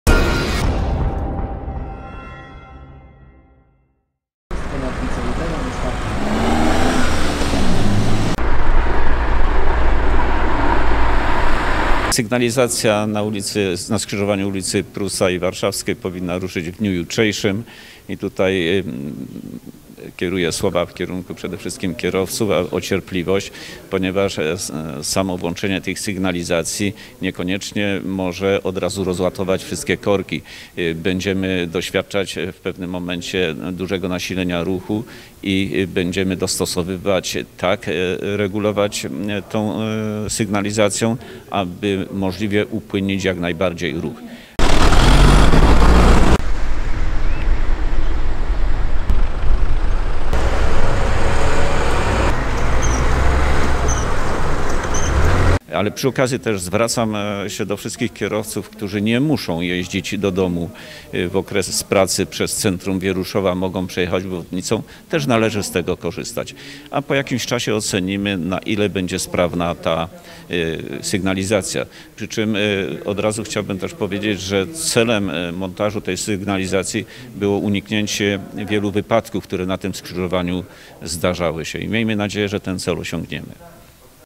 Sygnalizacja już na stałe od 31 maja
Od 31 maja (wtorek) już na stałe zostanie włączona sygnalizacja świetlna na skrzyżowaniu ul. Warszawskiej z ul. Fabryczną w Wieruszowie.

Informację potwierdził w rozmowie z portalem tugazeta.pl starosta powiatu wieruszowskiego Stefan Pietras. Włodarz powiatu ma jednocześnie apel do wszystkich kierowców. Zachęcamy do obejrzenia materiału filmowego.